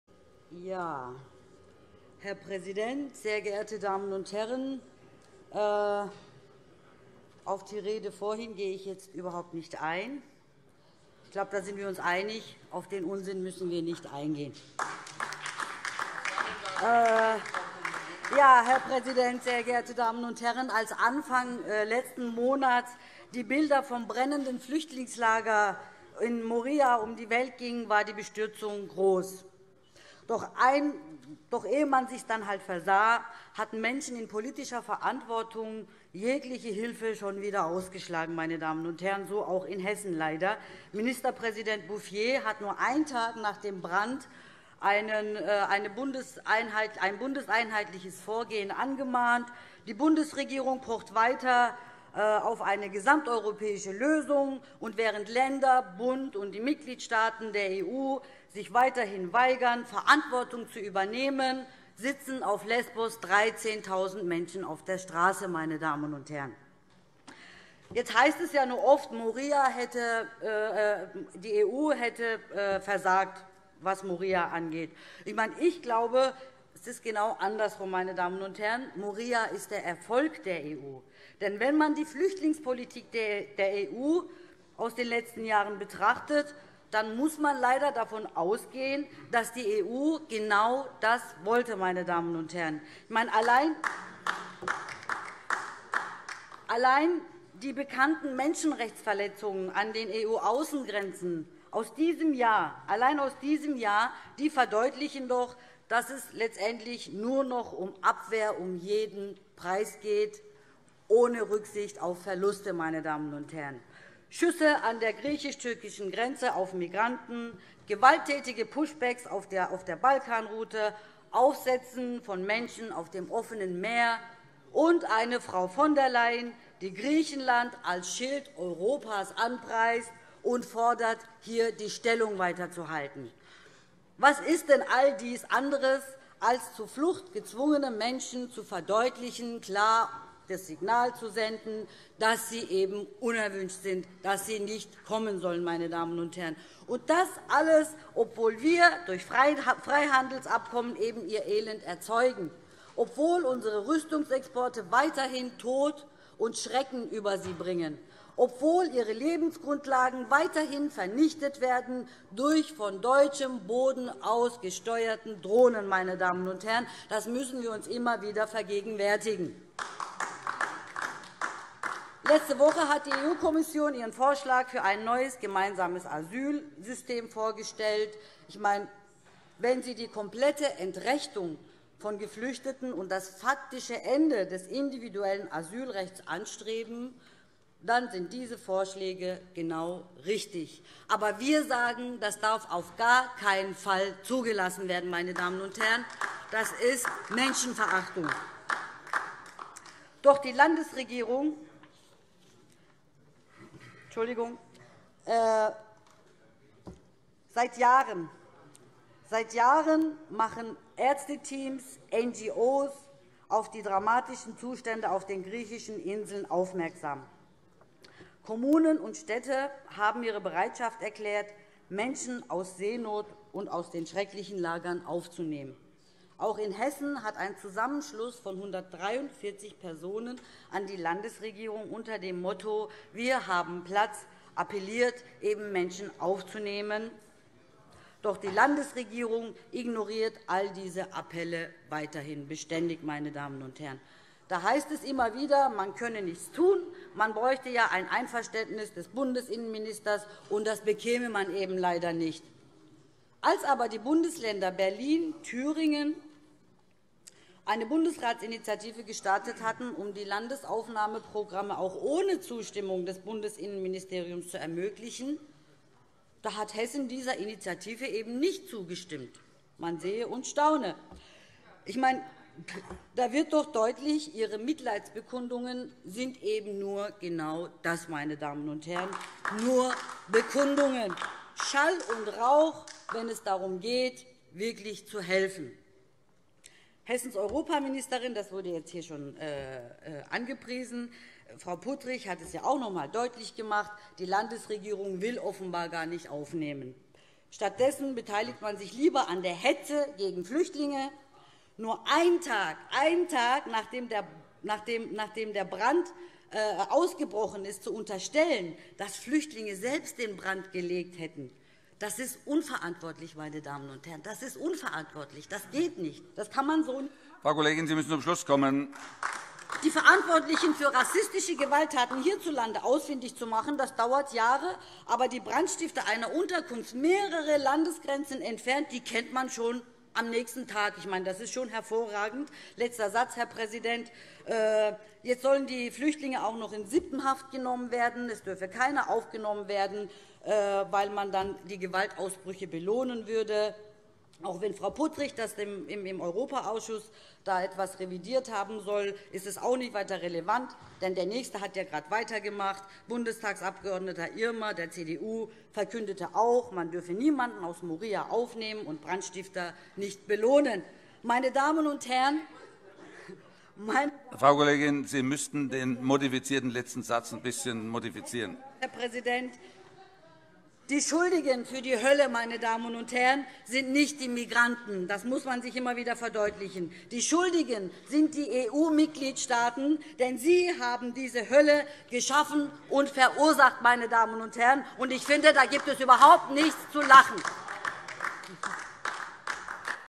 Saadet Sönmez - Hessen soll Menschen aus Moria mit einem Landesaufnahmeprogramm aufnehmen
In seiner 55. Plenarsitzung am 1. Oktober 2020 diskutierte der Hessische Landtag auf unseren Antrag hin über die Situation der geflüchteten in Moria. Dazu die Rede unserer migrationspolitischen Sprecherin Saadet Sönmez.